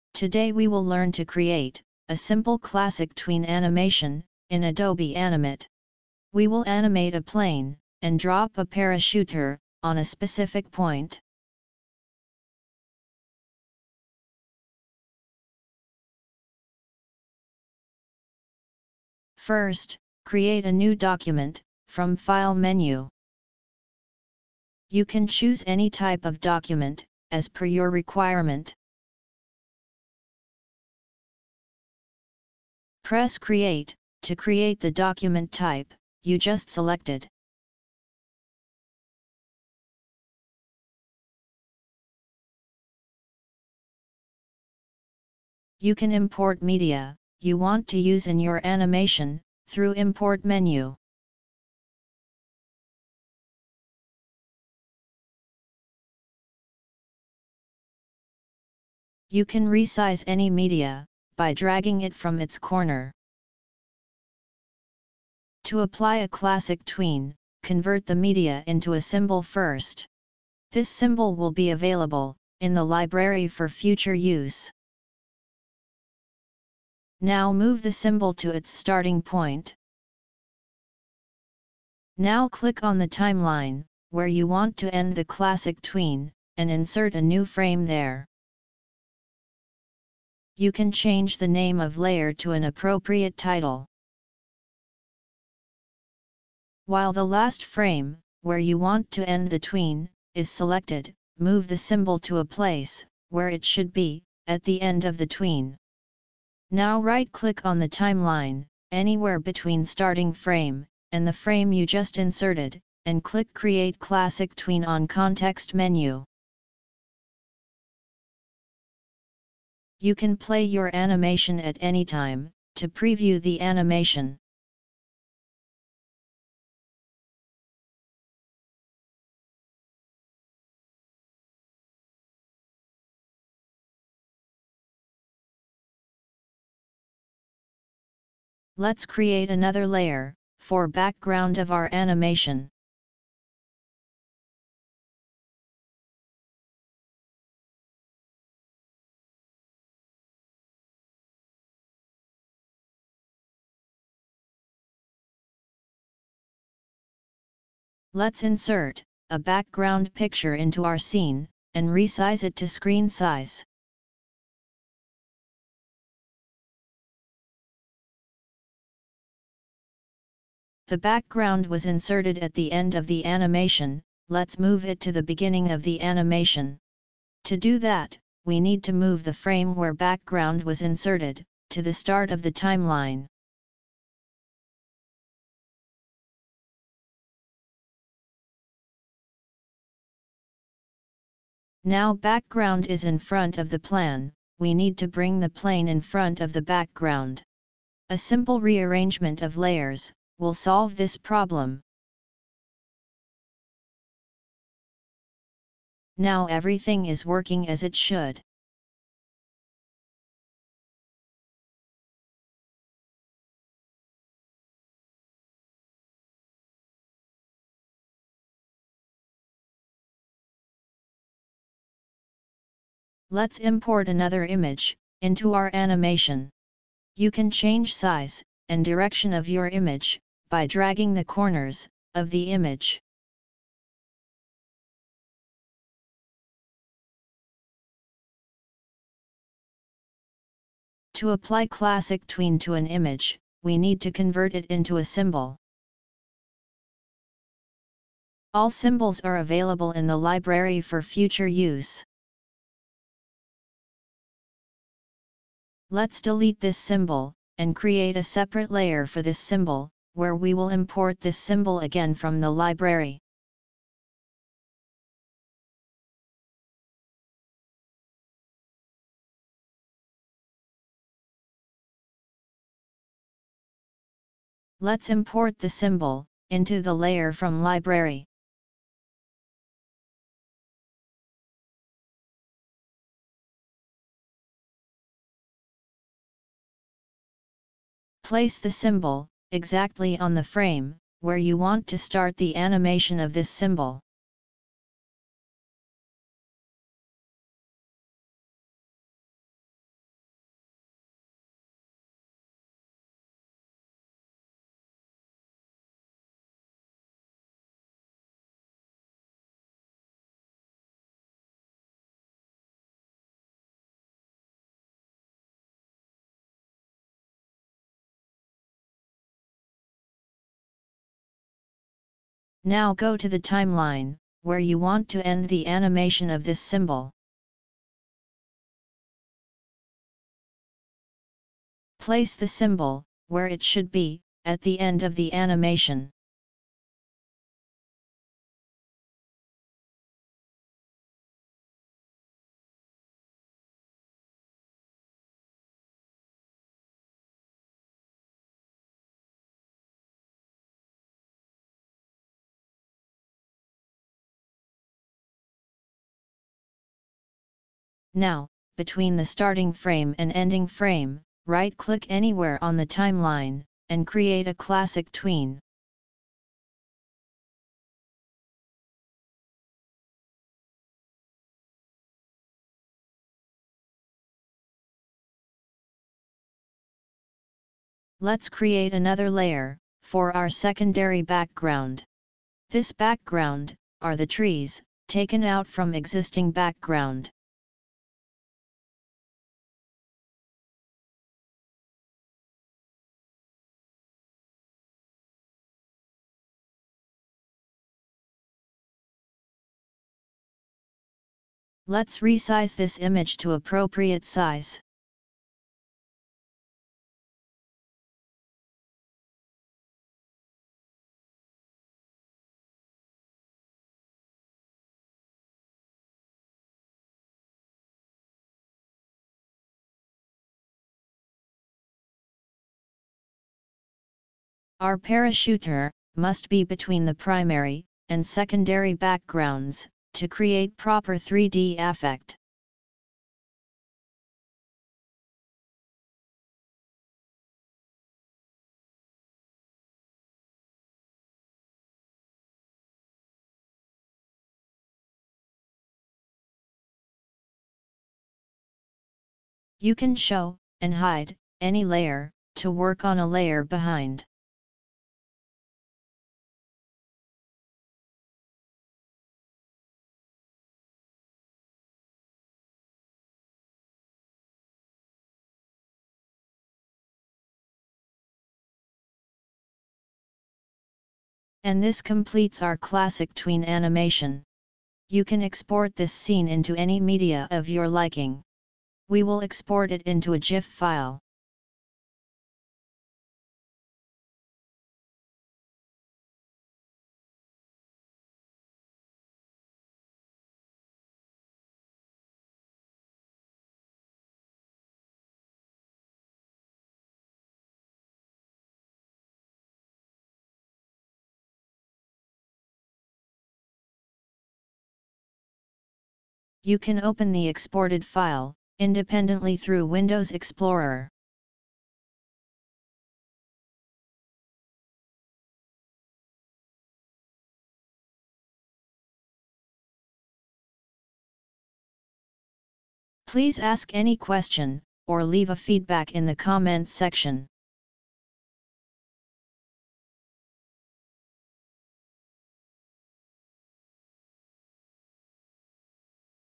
Animate: Classic Tween in Adobe Animate, parachuting from c130 (Animation Tutorial)
In this video we will show you how to create classic tweens in Adobe Animate. Create a flying plane animation. Create a dropping parachuter animation. Create a 3d effect in the animation. Export the animation to GIF file and run independently.